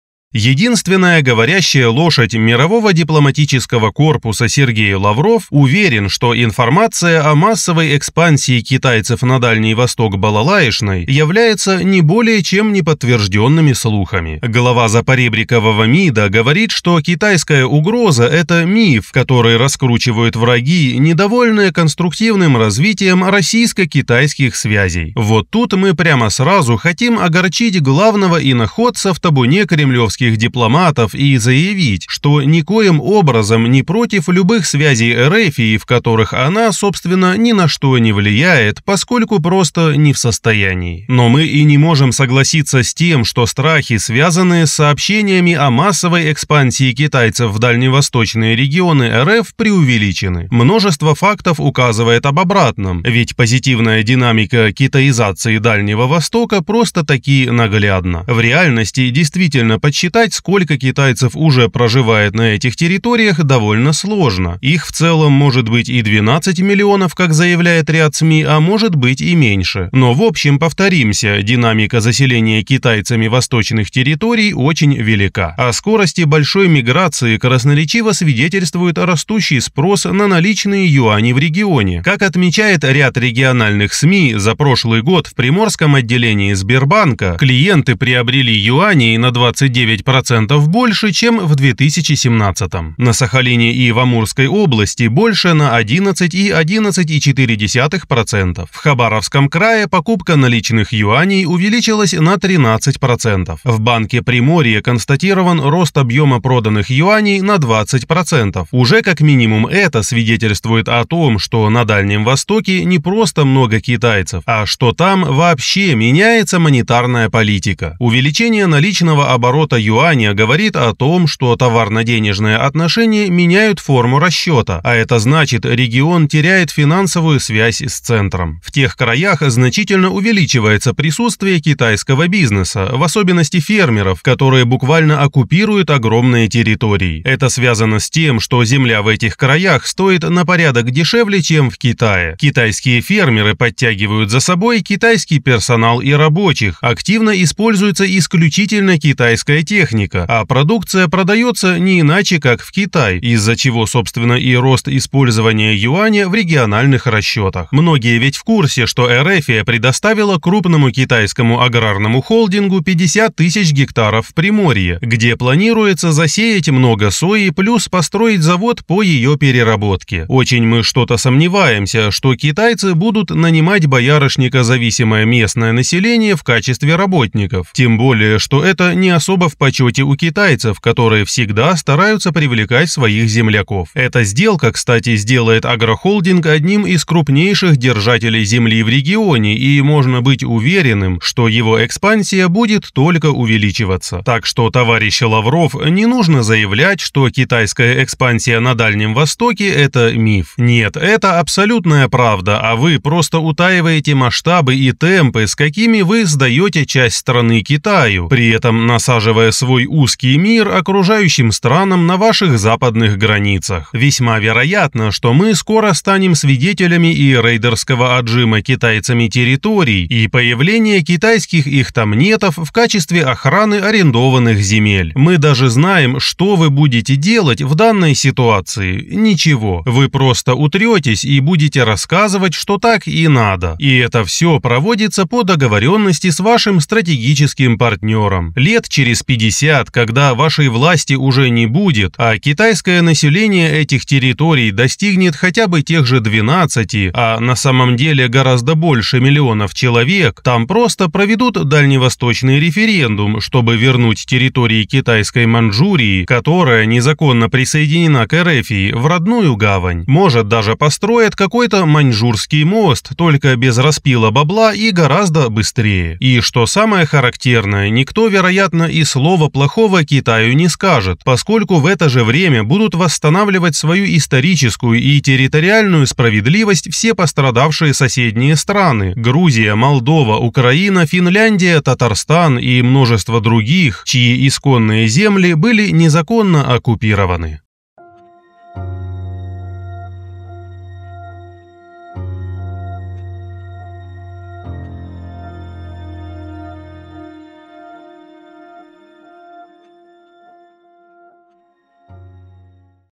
Лавров не верит в миллионы китайцев на Дальнем Востоке
Автор: der_Weide